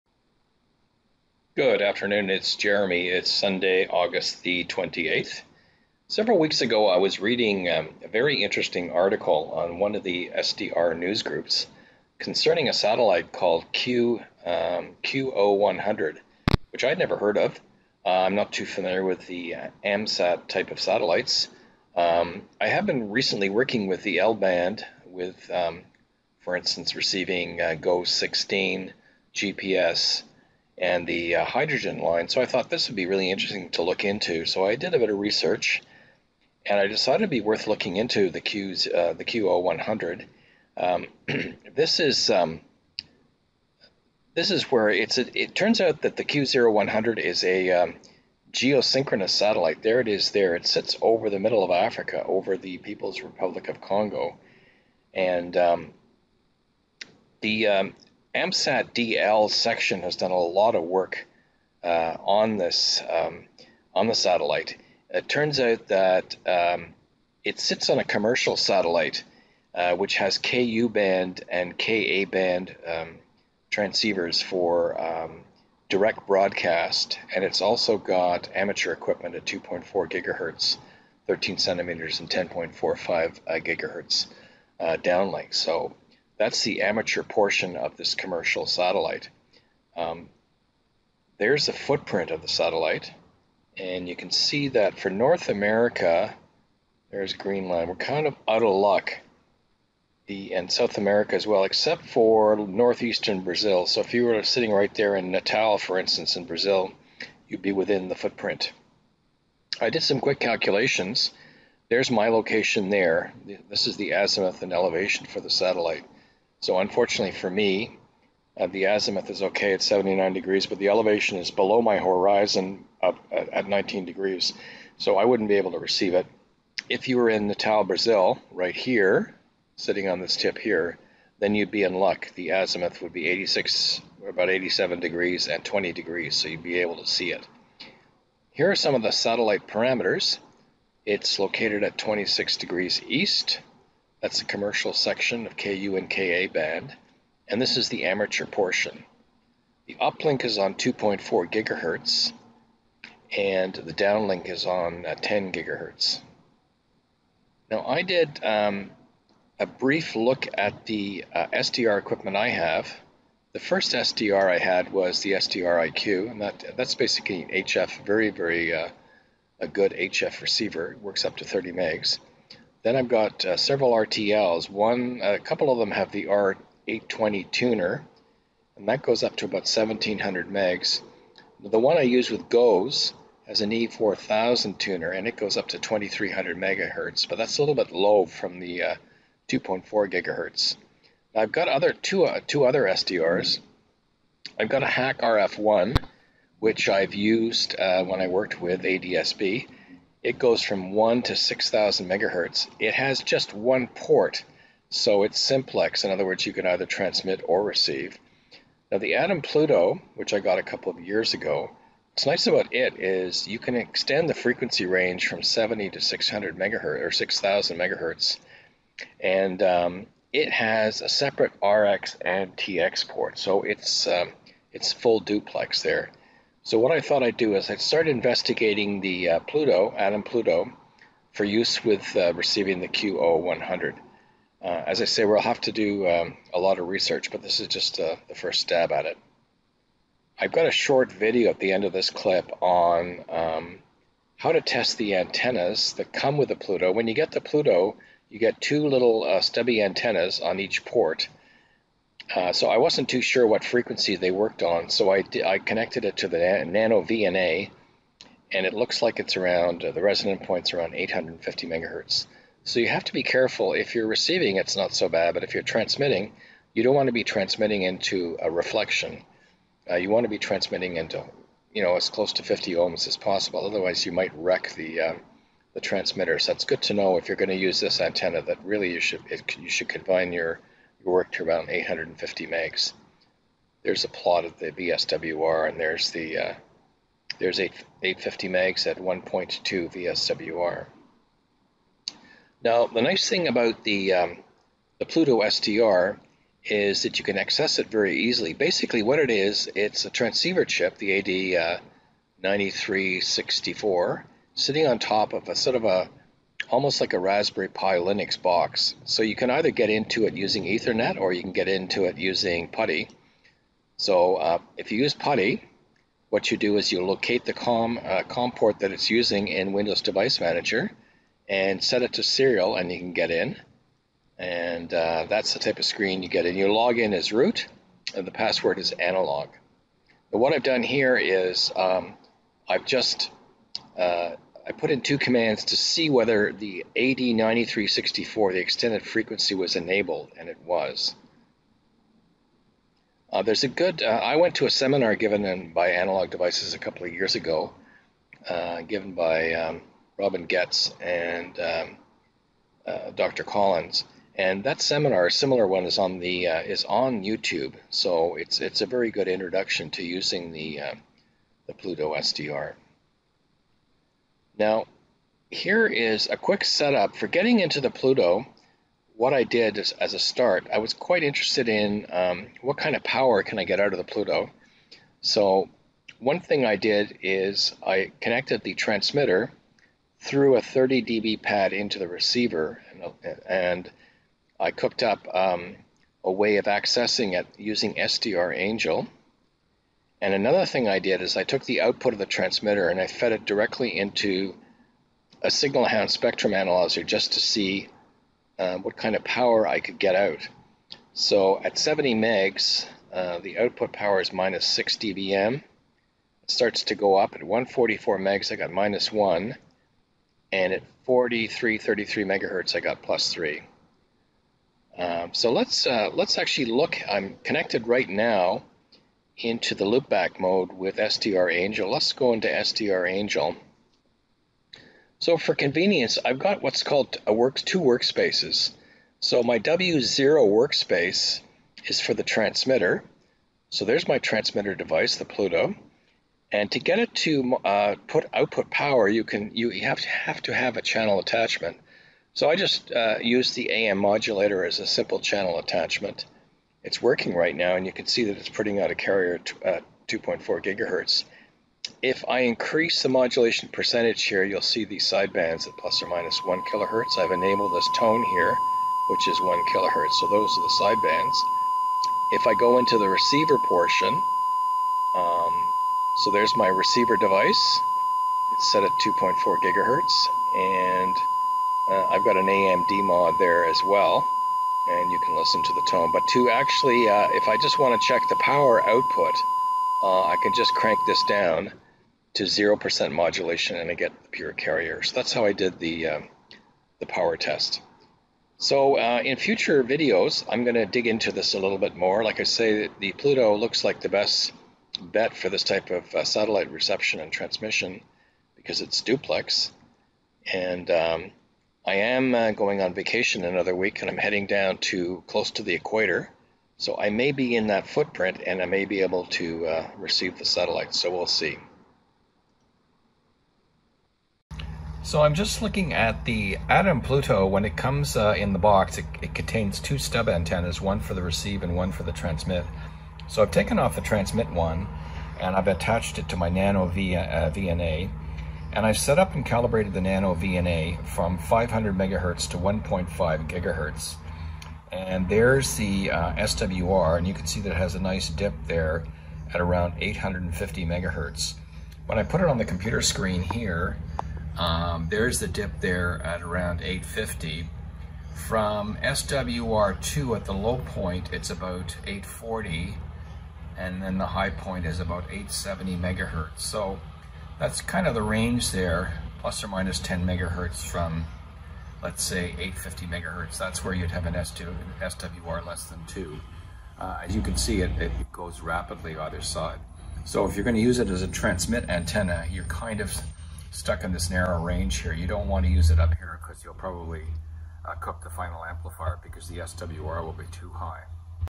SDR for QO-100 Satellite Reception - Planning
The QO-100 is an amateur radio geosynchronous satellite with 2.4GHz uplink and 10.45GHz downlink. Looking at various SDRs, the Pluto-SDR looks like it may be a good candidate for this task. Preliminary investigation of satellite parameters and hardware configuration.